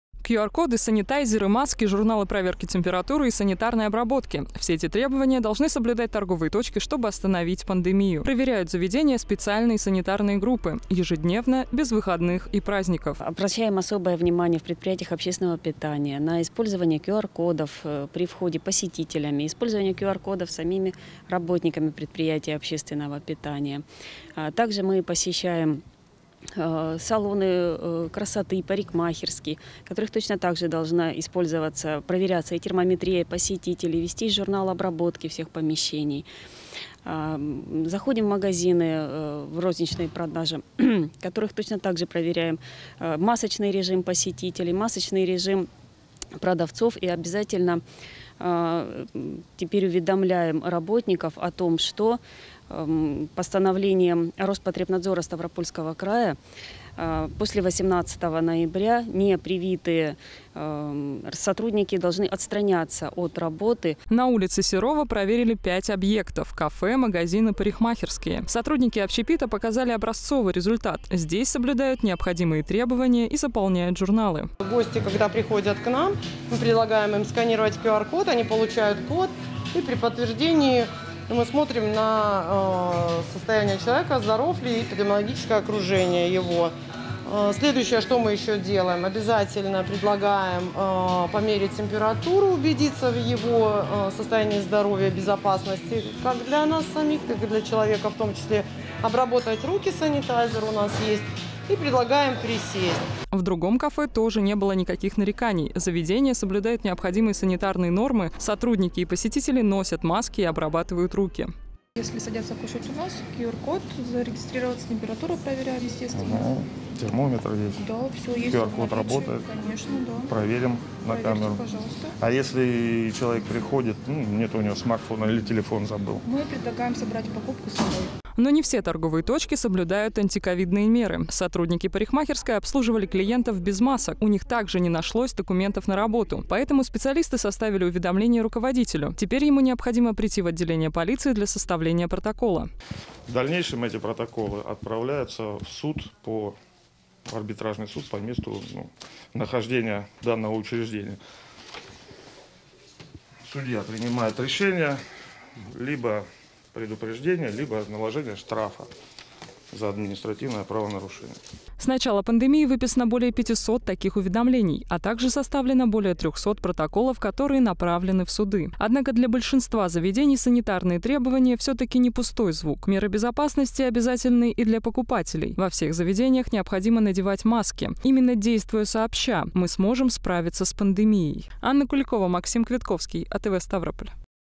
Масочный режим проверили в торговых точках Ставрополя
Qr-коды, санитайзеры, маски, журналы проверки температуры и санитарной обработки  – все эти требования должны соблюдать торговые точки, чтобы остановить пандемию.  Проверяют заведения специальные санитарные группы - ежедневно, без выходных и праздников.